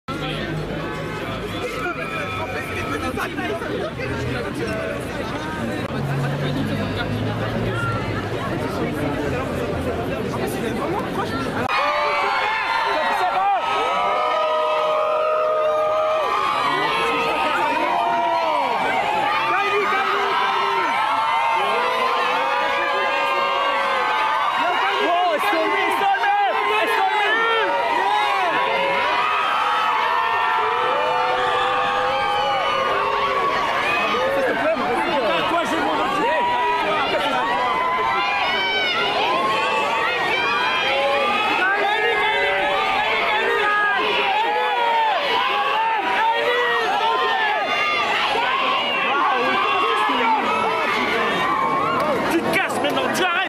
Kylie Jenner & Stormi - Valentino Haute Couture fashion show in Paris - 24.01.2024
American socialite Kylie Jenner with her daughter Stormi attending the Valentino Haute Couture Spring/Summer 2024 fashion show in Paris, January 24th 2024.